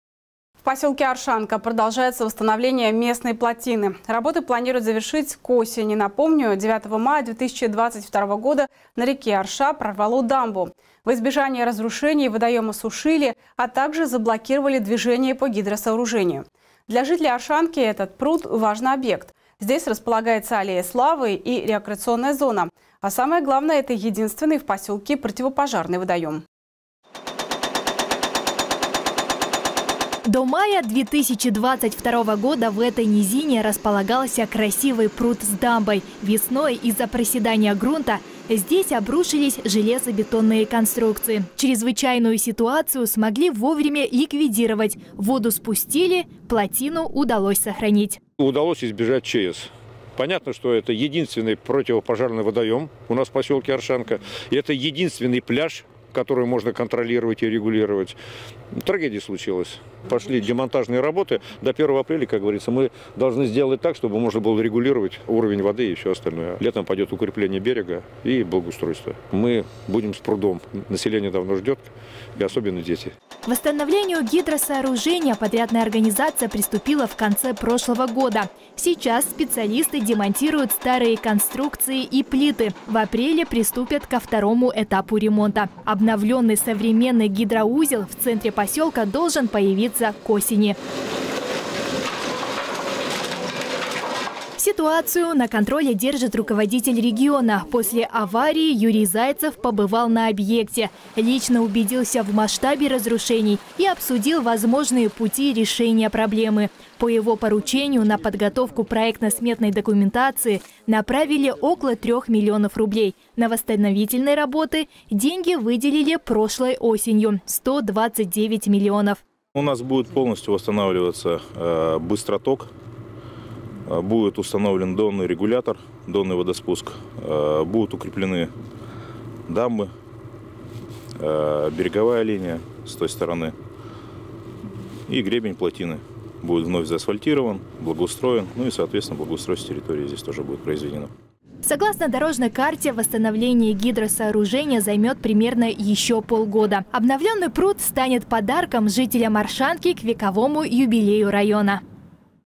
В посёлке Оршанка продолжается восстановление местной плотины
Работы планируют завершить к осени. Напомним, 9 мая 2022 года на реке Орша прорвало дамбу. Во избежание разрушений, водоём осушили, а также заблокировали движение по гидросооружению. Для жителей Оршанки этот пруд – важный объект. Здесь располагаются аллея славы и рекреационная зона, а самое главное, единственный в посёлке  противопожарный водоём.